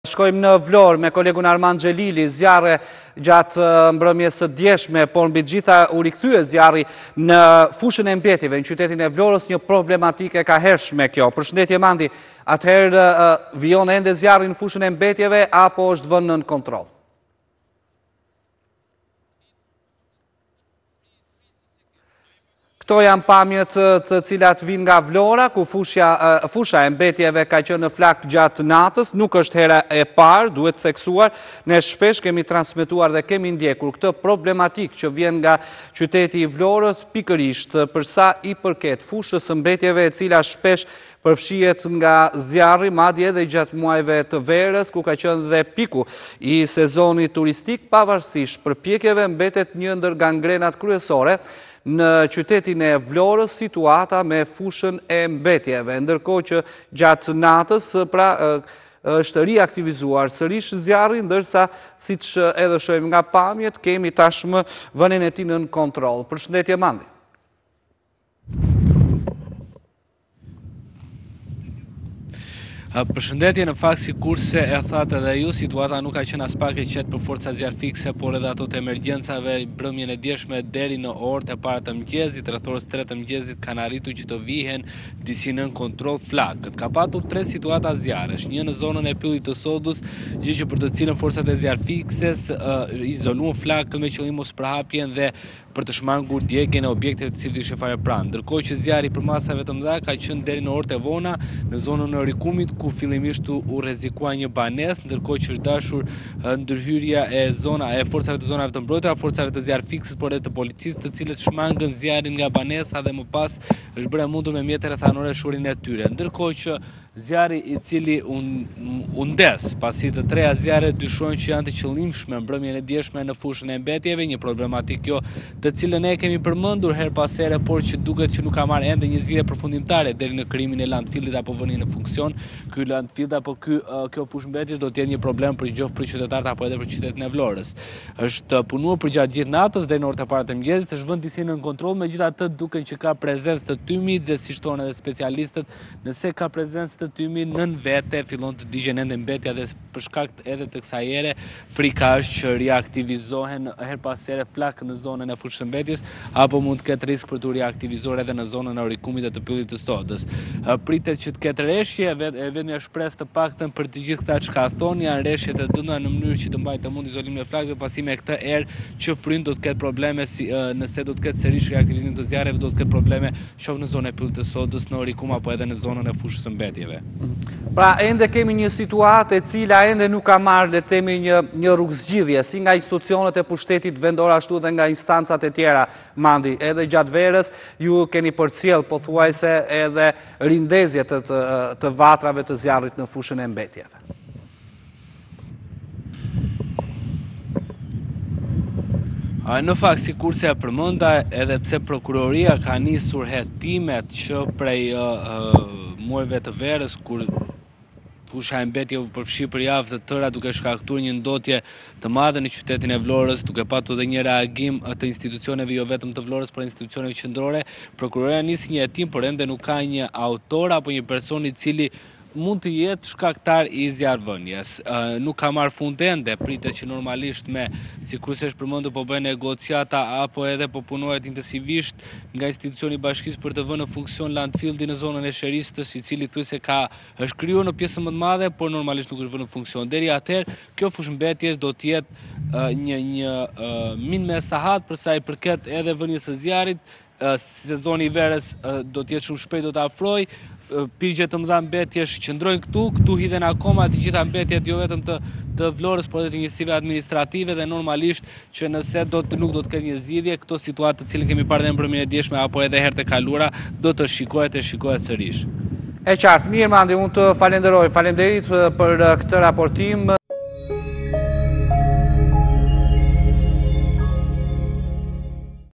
Vlorë/ Përfshihet nga flakët gjatë natës fusha e mbetjeve, zjarri vihet në kontroll
Fusha e mbetjeve të qytetit të Vlorës është përfshirë nga flakët gjatë natës. Flakët kanë qenë të përmasave të mëdha, ndërsa zjarrfikësit kanë shkuar në vendin e ngjarjes për të bërë të mundur shuarjen e tyre.